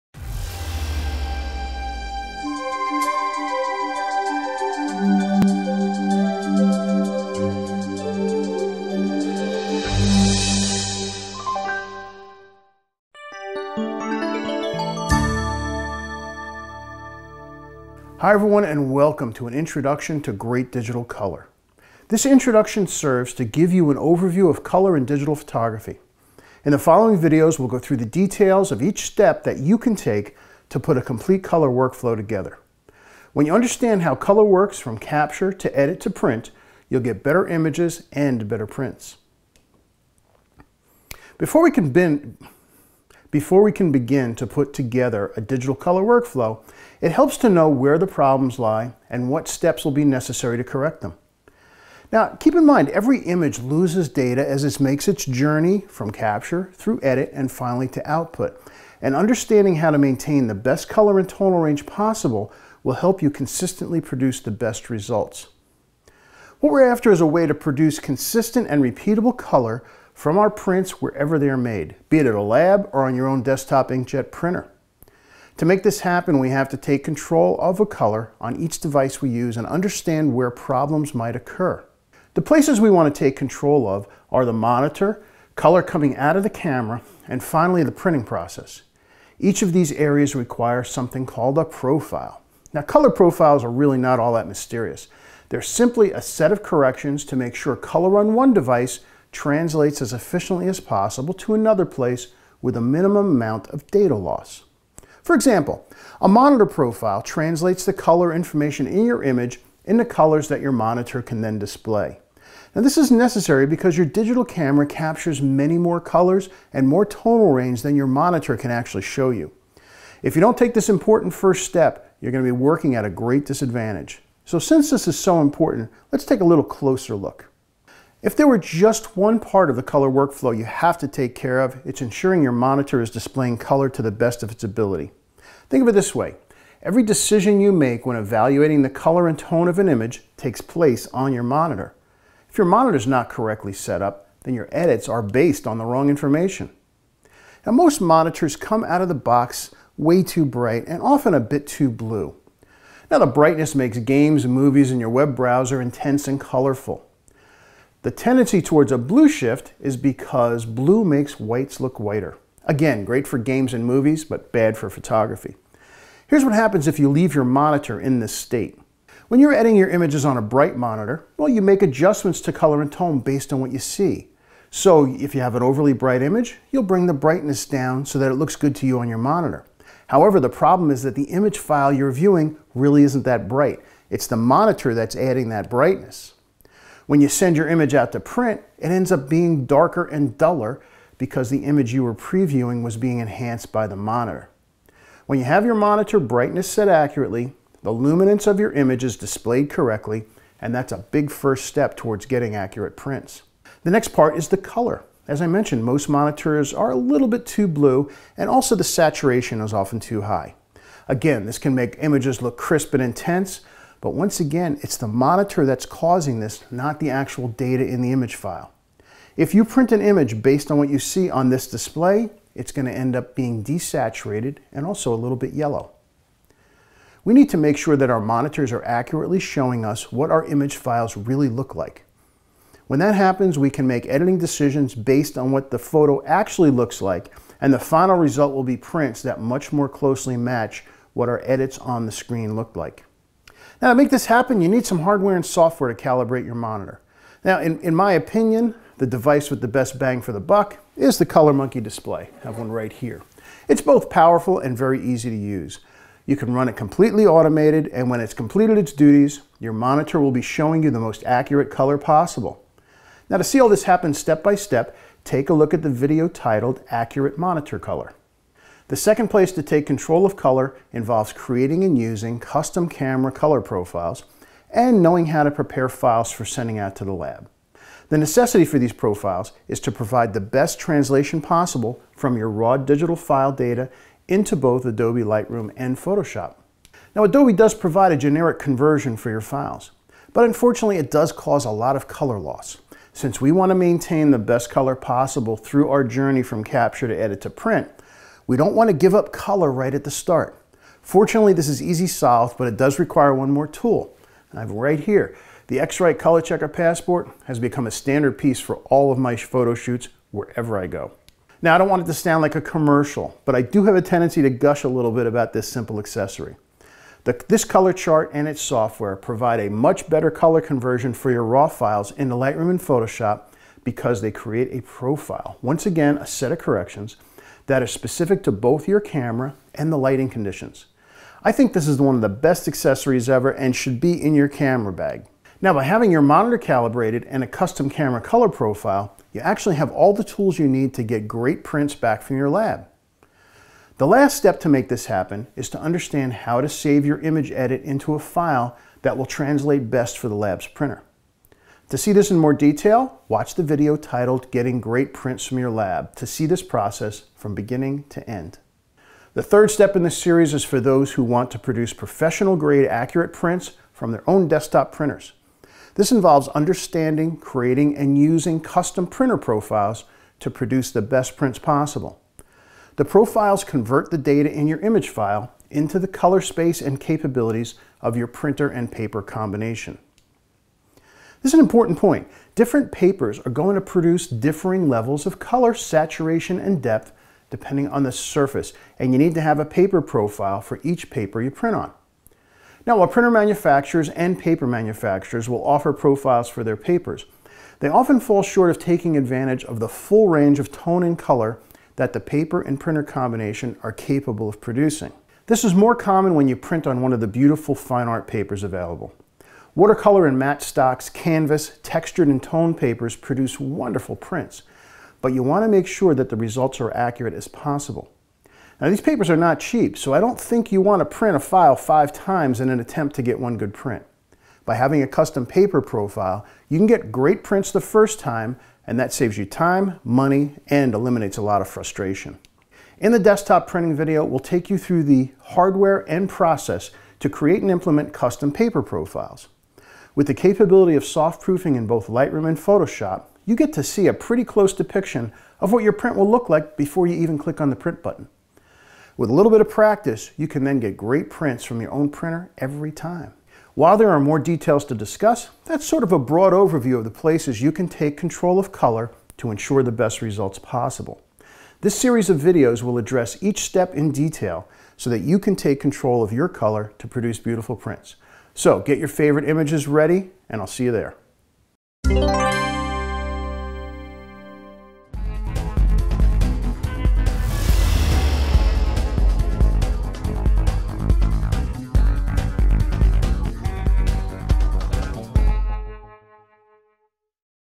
1 Intro to Digital Color
In this new series on Digital Photography Color Workflow Joe covers the How & Why of color from capture to edit to print. Part one introduces the concepts and issues so that you have an understanding on how to get started towards great print results every time!